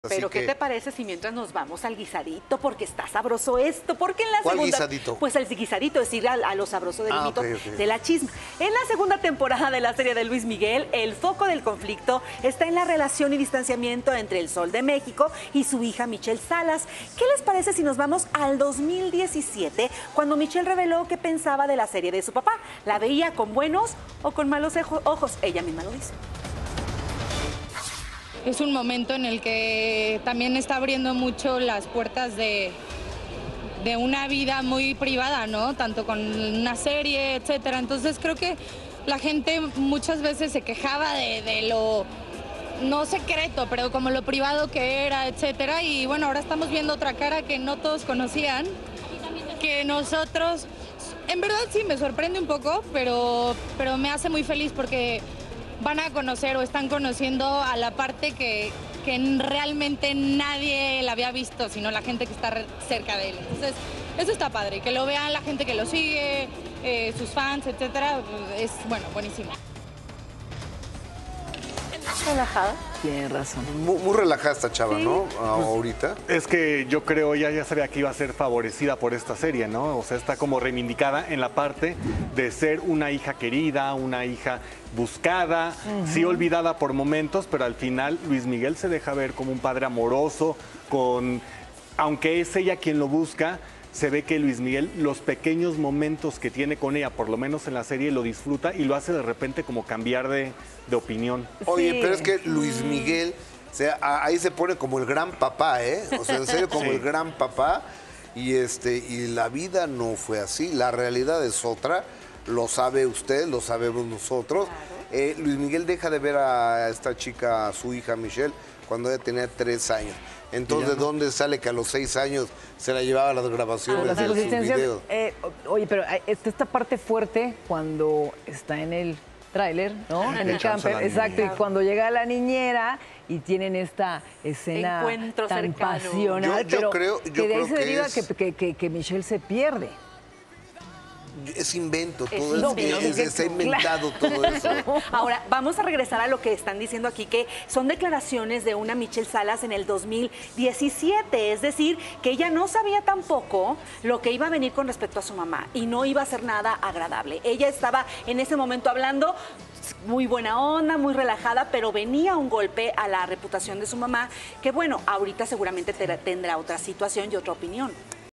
Lo que Michelle Salas dijo sobre la serie de su papá Luis Miguel | De Primera Mano
Esta fue la opinión de MichelleSalas sobre la serie de su padre LuisMiguel antes de que se estrenara...